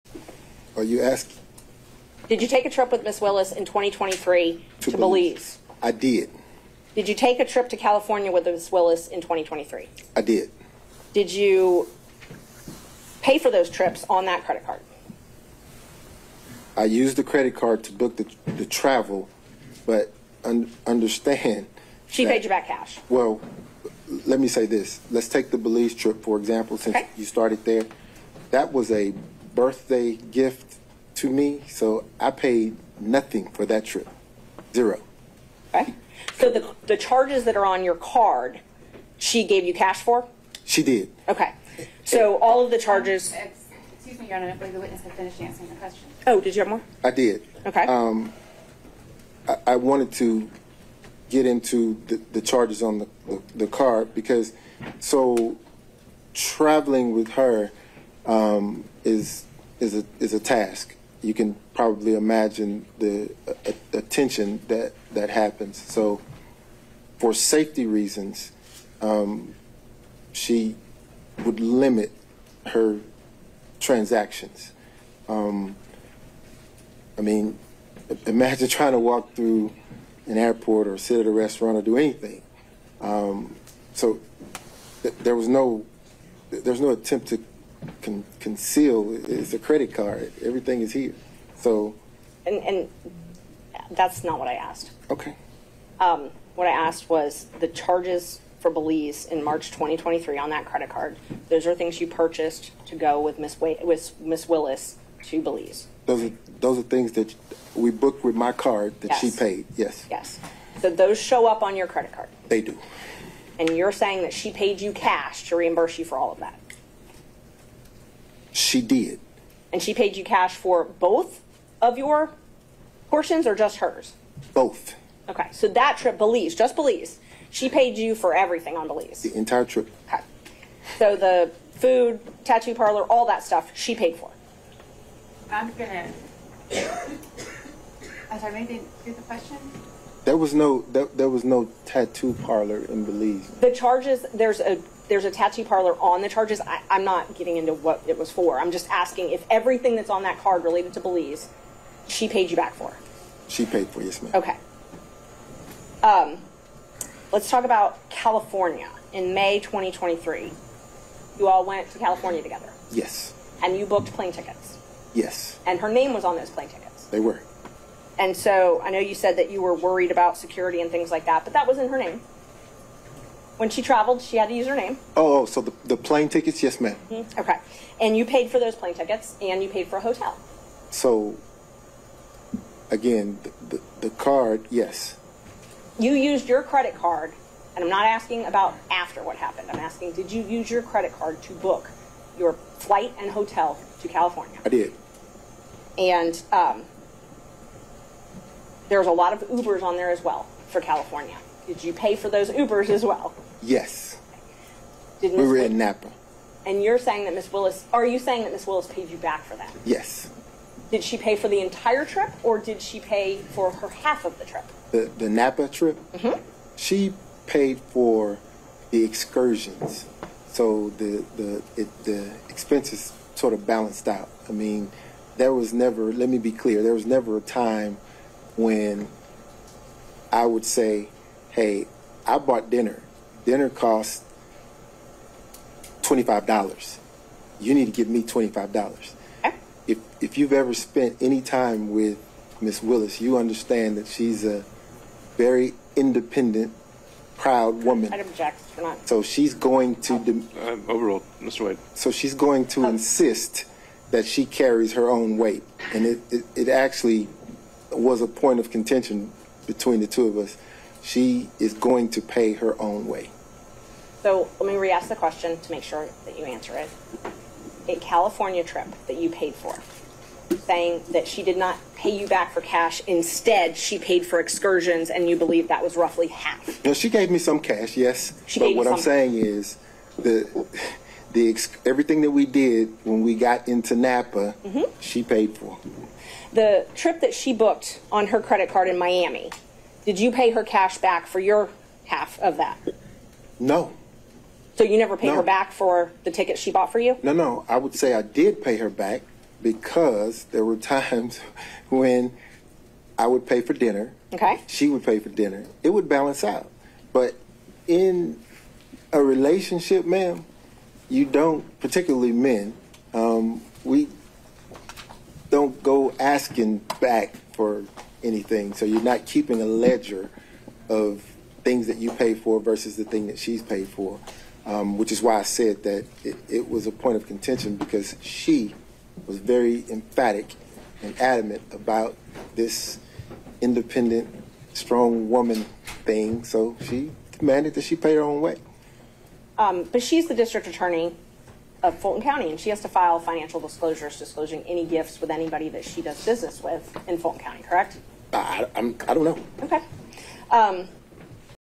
Nathan Wade talks about trips he and Fani Willis went on in court
Wade took the stand in a court hearing deciding if he and Willis will stay on Donald Trump's RICO case in Georgia.